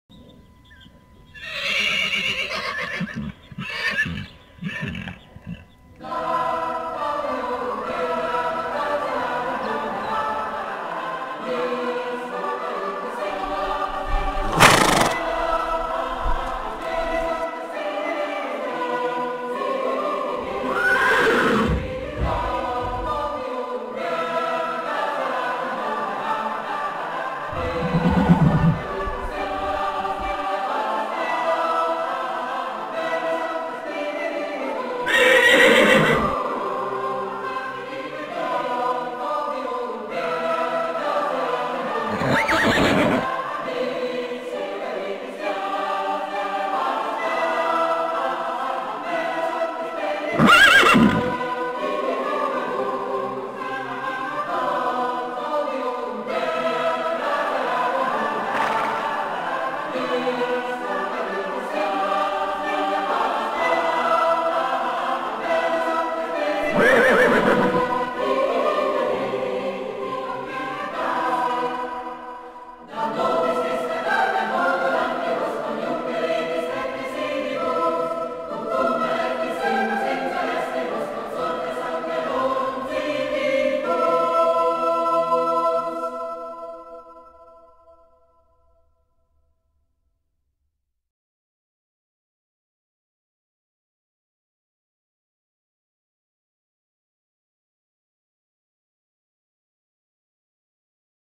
Equine Painting. Horse Painting. Chinese Horses.
A Video displaying some paintings on Chinese Horse Painting. Horses are difficult for me. Chinese Horses ever more so. People love Horse Paintings and I hope this one inspires anyway. t is about the first time I have ever painted anything equine. So lets hope.